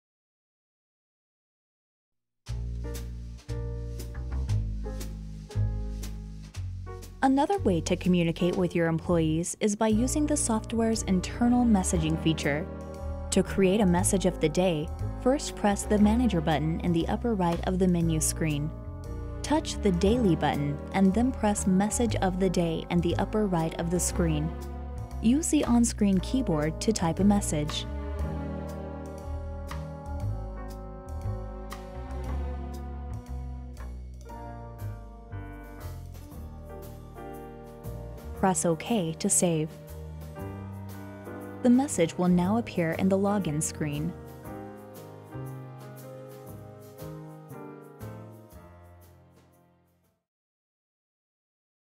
Restaurant Mgr Sec18 Message of Day
Harbortouch POS system restaurant tutorial on how to add a message of the day for restaurant managers.